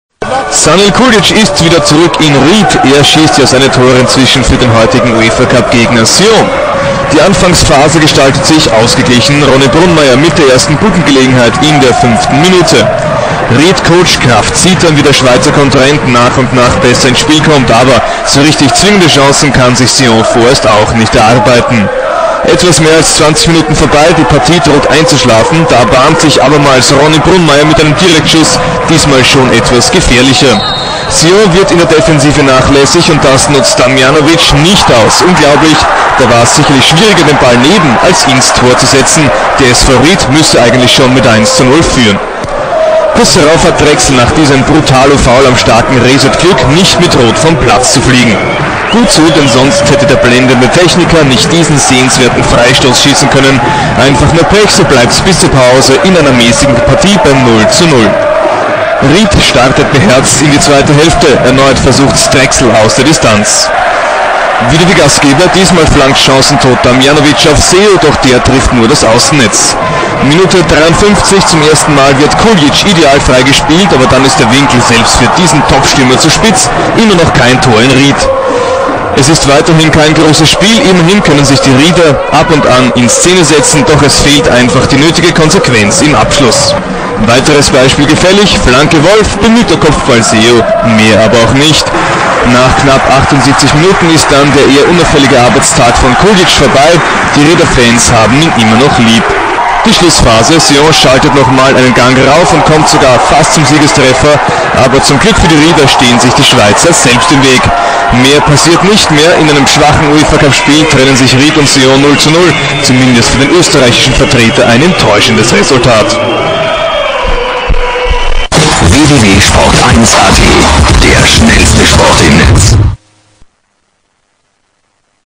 2006.8.10 UEFA CUP Q2 SV Ried-FC Sion.avi (Ried Home)
2006/07 UEFA Cup Q2 Ried Home 0-0